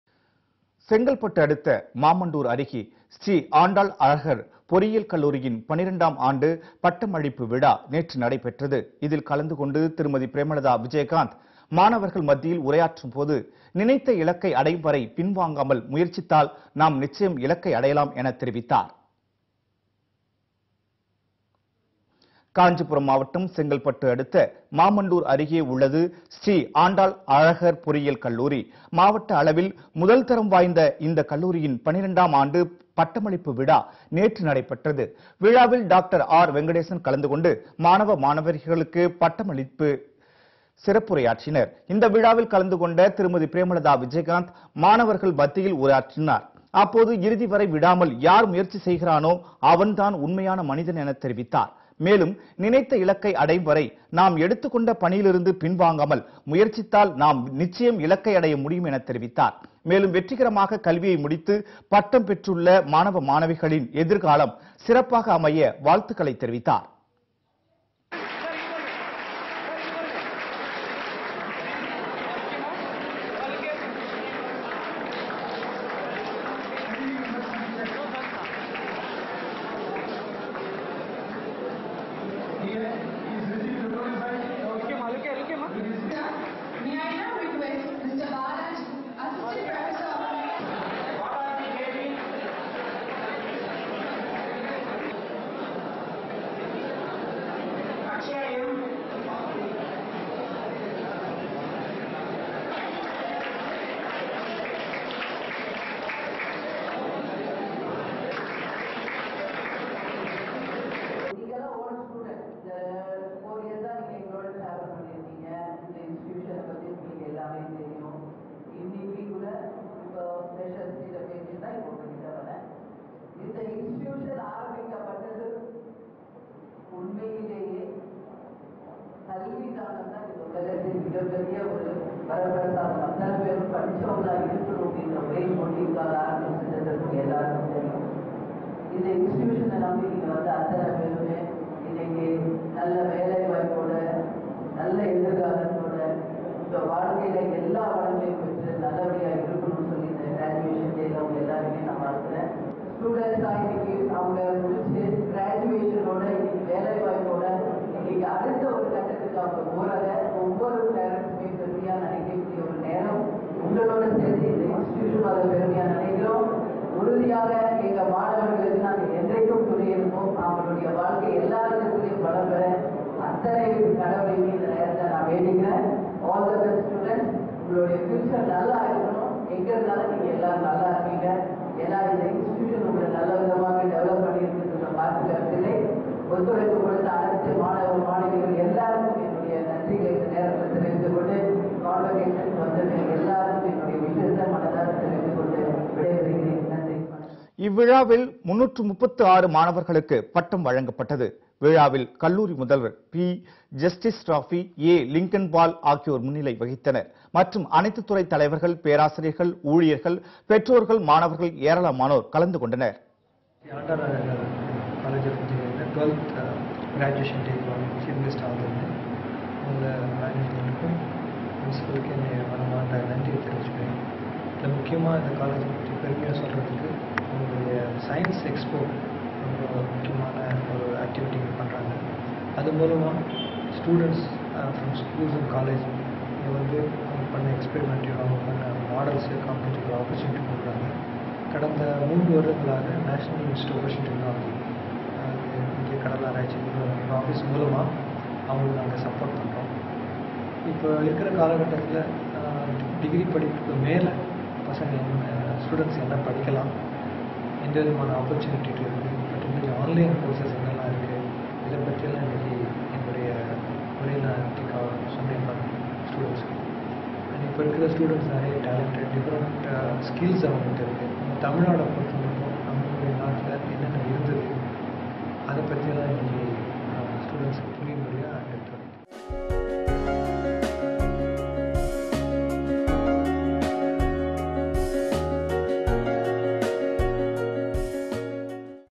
மாணவர்களிடையே திருமதி. பிரேமலதா விஜயகாந்த் தன்னம்பிக்கை உரை.
மாணவர்களிடையே திருமதி. பிரேமலதா விஜயகாந்த் தன்னம்பிக்கை உரை | Captain News | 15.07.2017